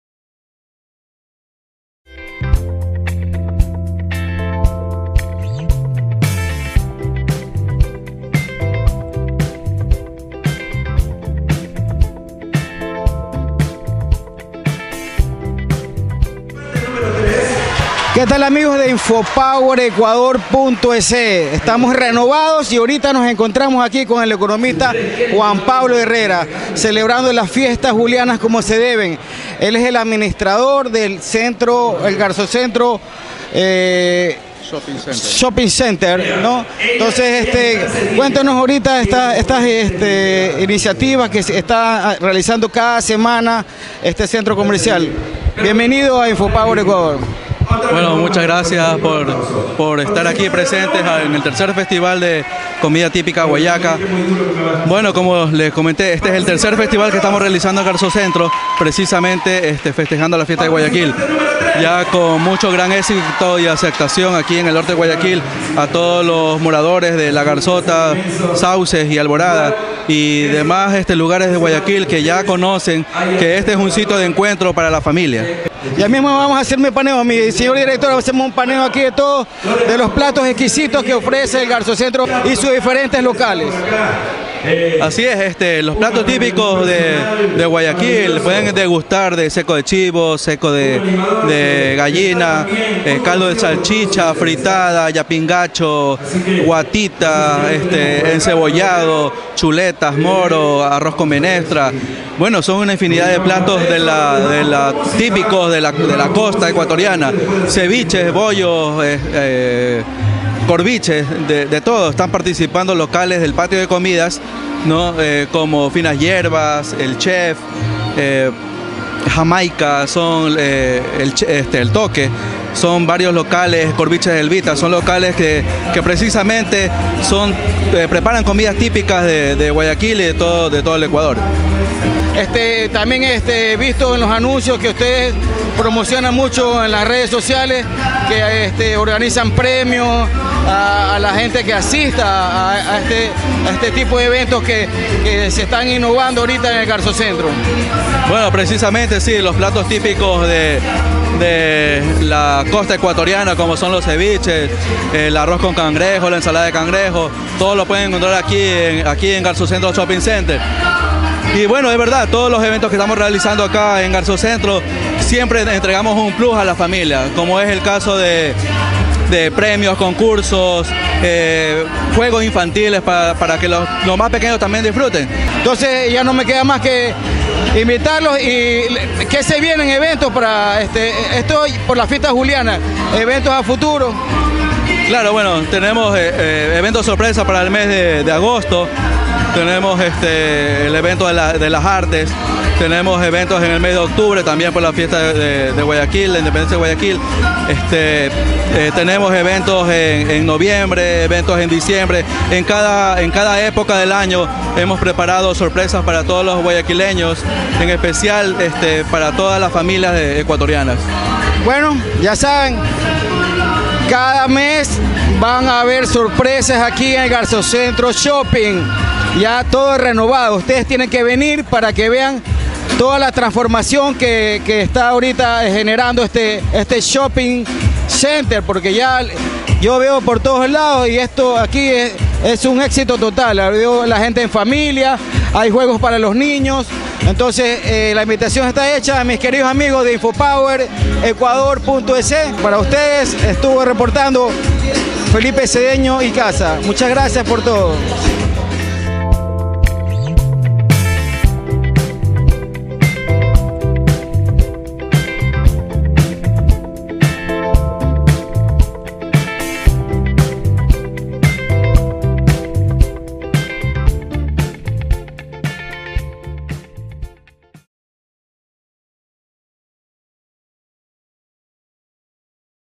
3era Feria Gastronómica Garzocentro Shopping Fiestas Julianas
Hoy se inició el Tercer Festival de Comida Típica Guayaca y estuvo todo listo para empezar la fiesta que se preparó con esmero para todo Guayaquil.